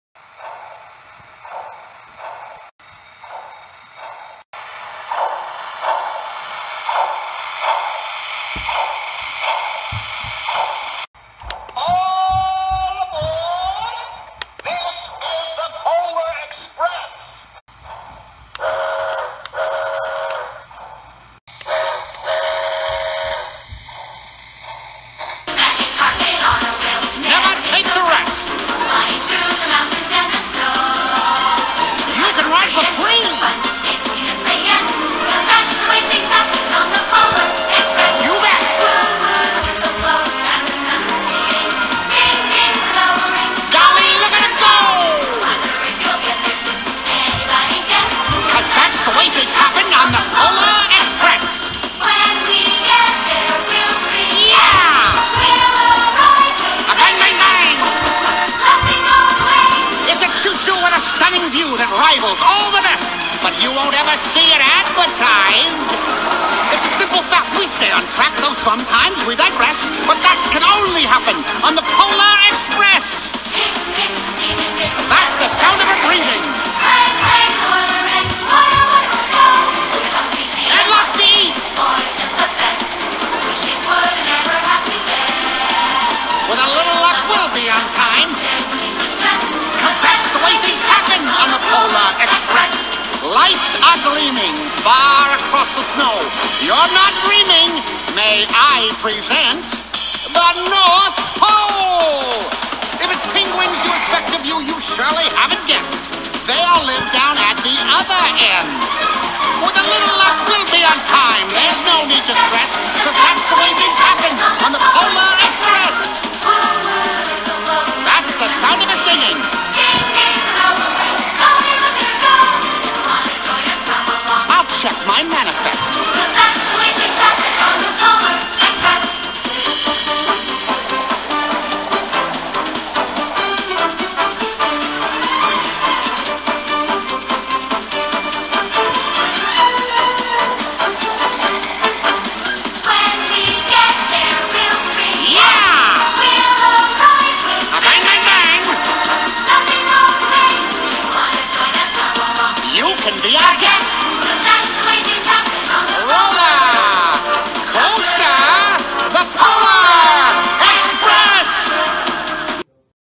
the polar express short film
I made this video because I got the bachmann polar express for christmas ever sence I got  it i have been playing with it but my dad said film the train running on the track so that is what I did thanks for watch click that subscribe button we are almost to 100 subs thanks for watching and have a great day!